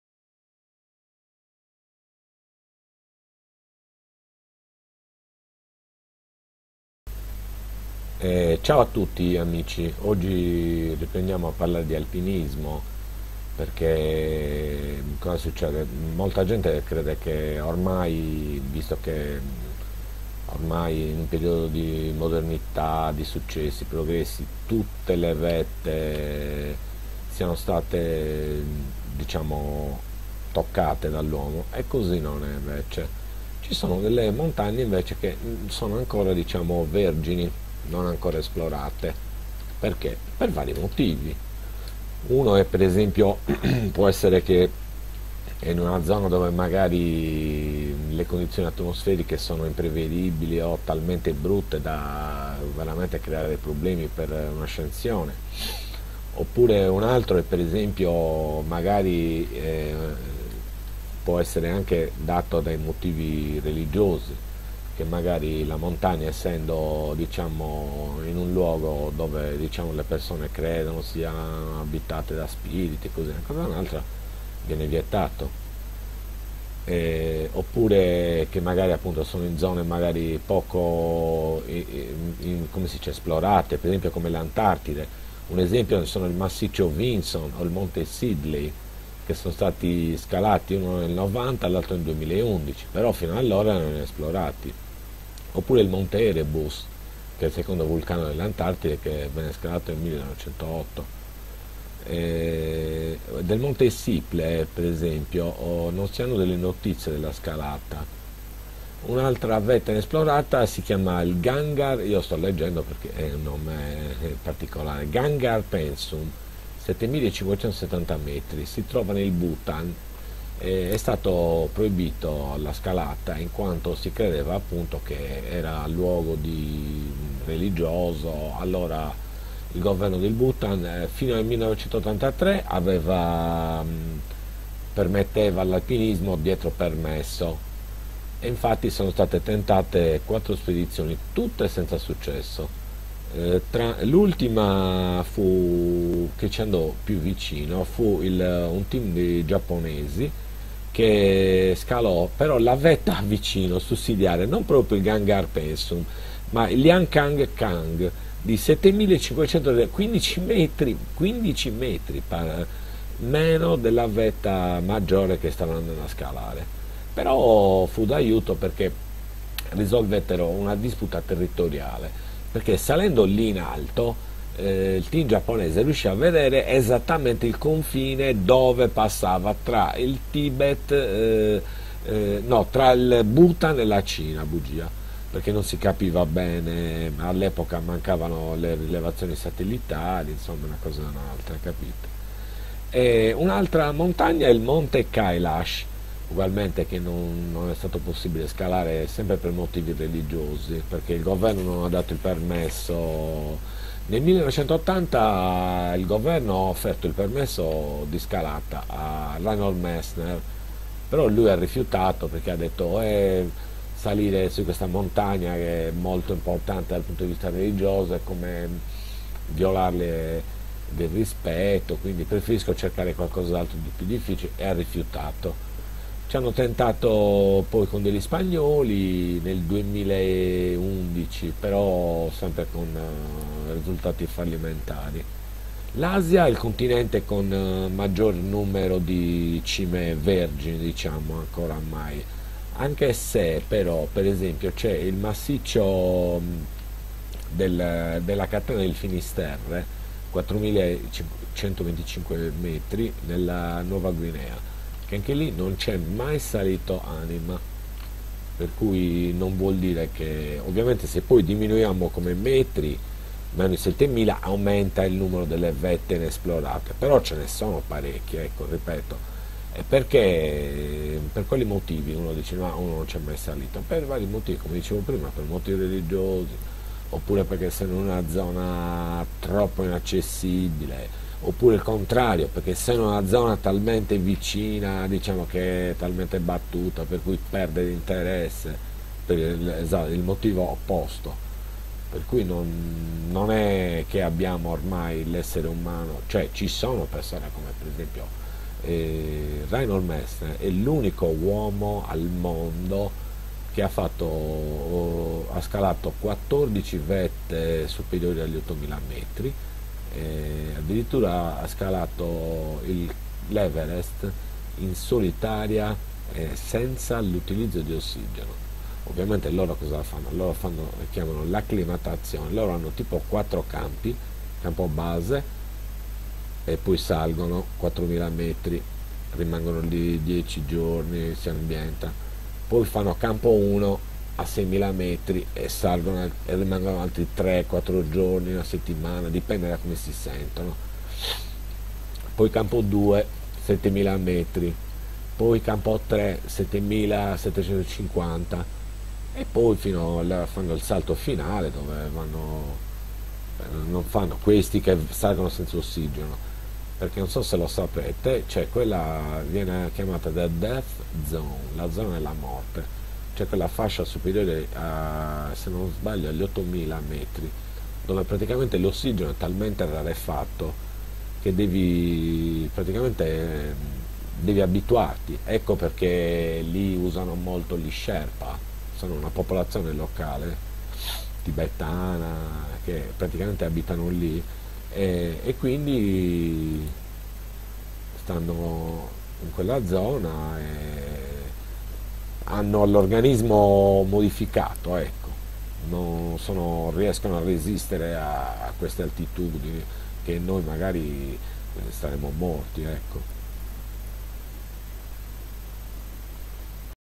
Montagne ancora inesplorate
in questo video riprendiamo il discorso sull'alpinismo perchè il pensiero comune porta a pensare che oramai si sia scalato tutte le montagne piu' alte del mondo ma non è cosi per svariati motivi alcuni di natura pratica alcuni di natura spirituale hanno portato alcune vette ad essere ancora inesplorate.